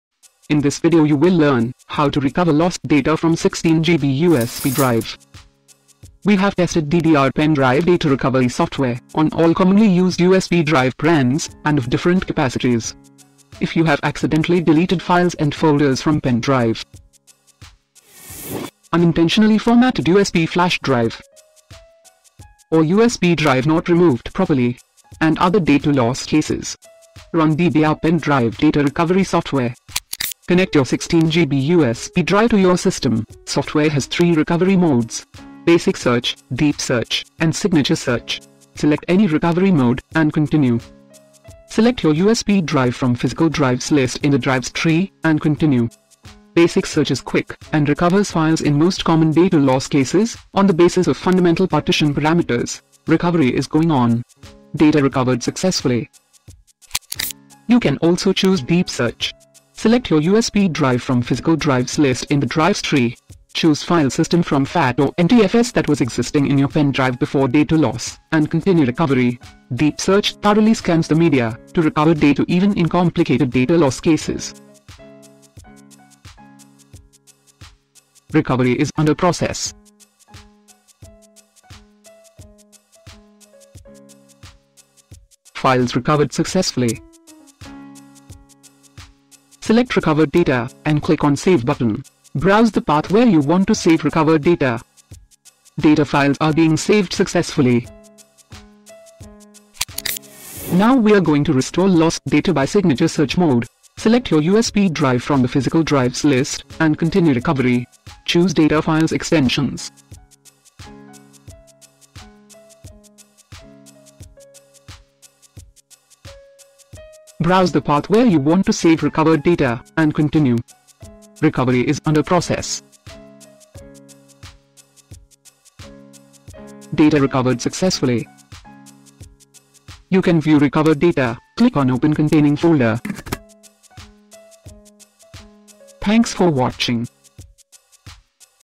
DDR Pen Drive Recovery Software: Recover data from 16GB USB Drive
In this video we have described process to recover lost files and folder data from 16GB USB Drive using DDR Pen Drive Recovery Software. Software facilitates to recover lost data saved in different file formats including JPG, PNG, GIF, BMP, Bitmap, WMF, TIFF, EXE, MP3, MP4, MPEG, MPG, PDF, DOC, PPT and more.